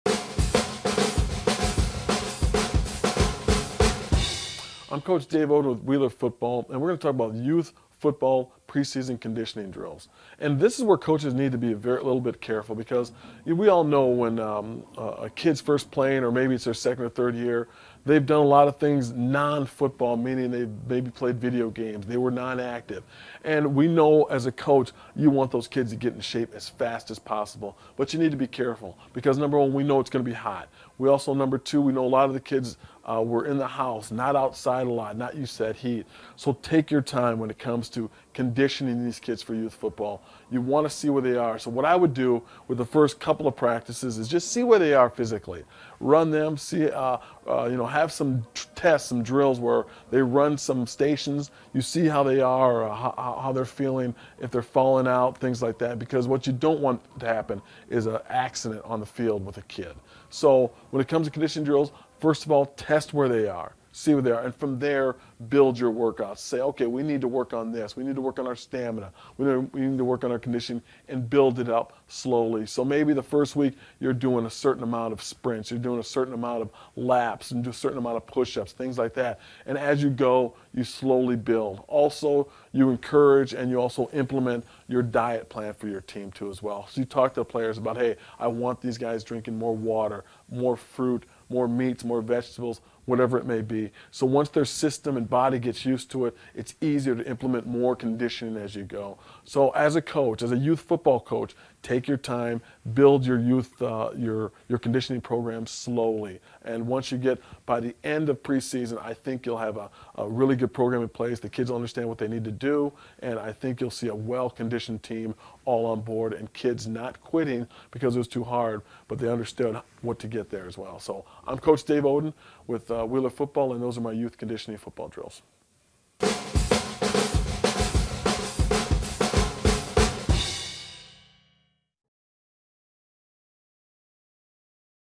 Youth Football Pre-Season Conditioning Drills
Youth Football Pre-Season Conditioning Drills. Part of the series: Youth Football. The pre-season is a time to learn football and increase your players' conditioning. Start the season off right with these drills presented by a professional youth football coach in this free video.